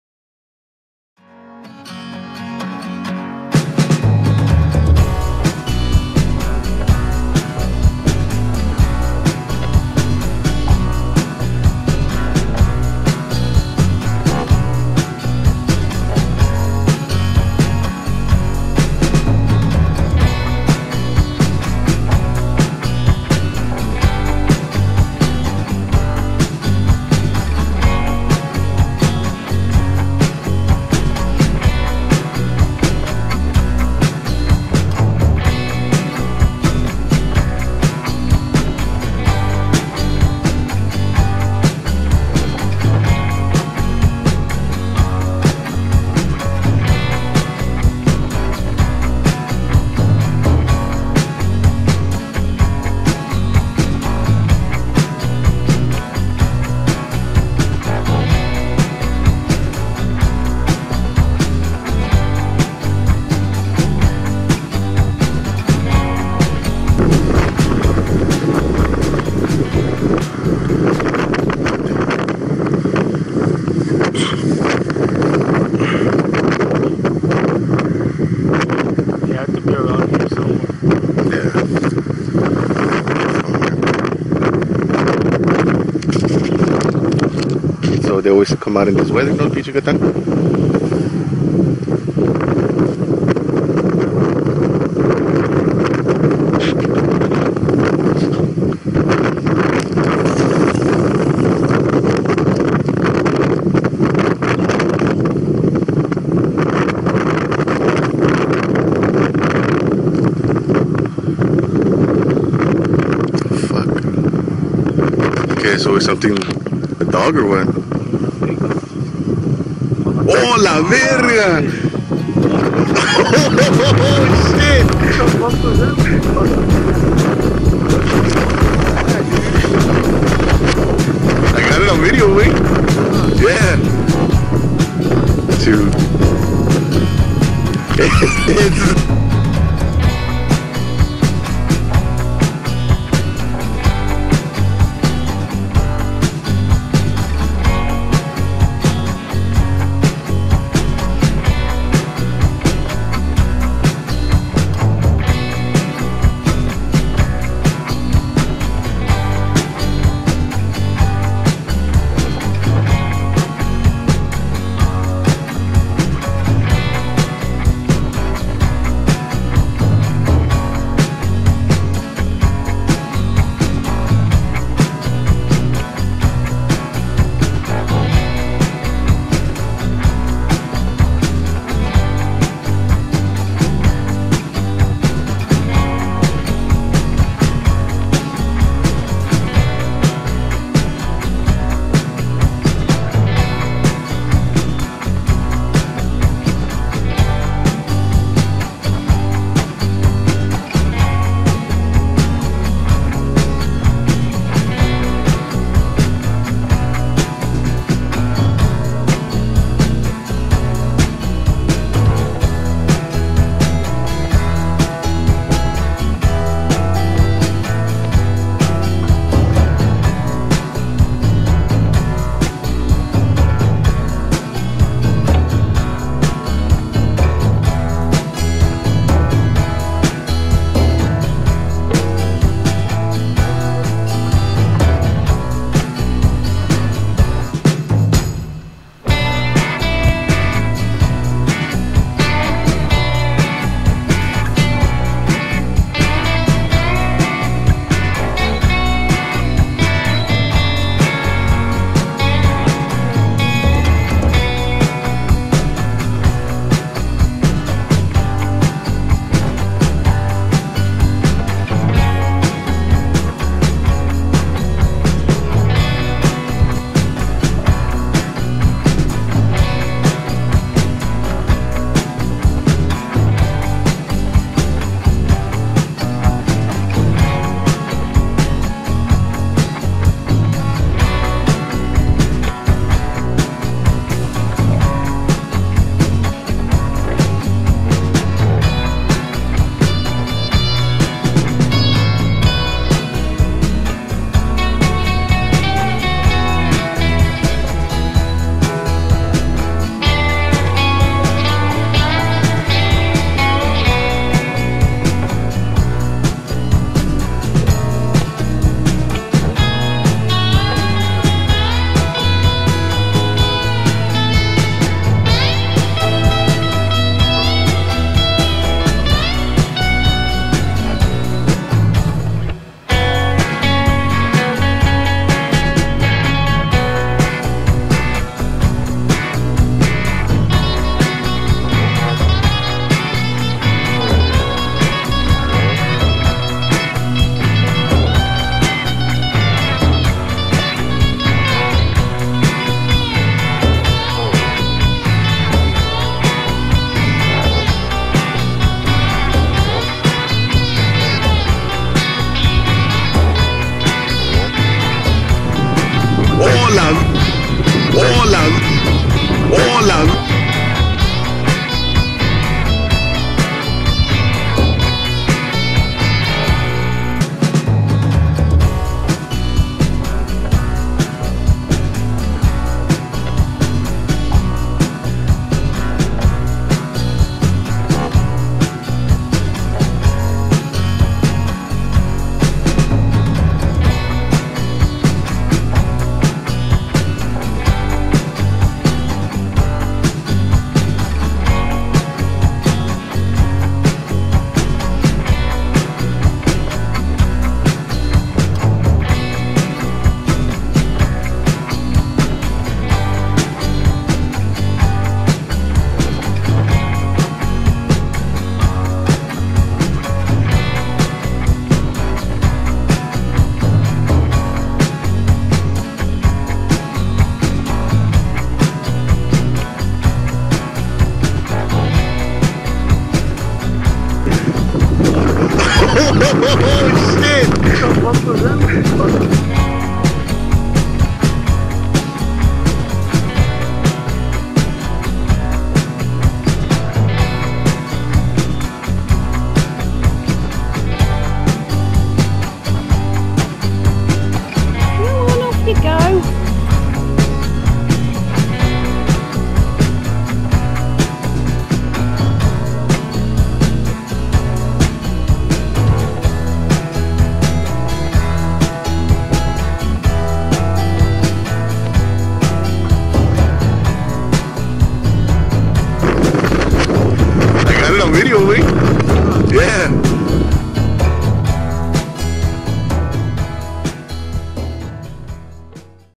SOUTH TEXAS - BIGFOOT OR DOGMAN?
Dog-like Humanoid Creature Caught On Camera
As featured on Bigfoot / Cryptids / Dogman  - July 19, 2019

Two men were fishing for Alligator Gar near the United States - Mexico border when they spotted a large unusual creature at the water’s edge. They first thought it was a dog, until it stood up on its hind legs.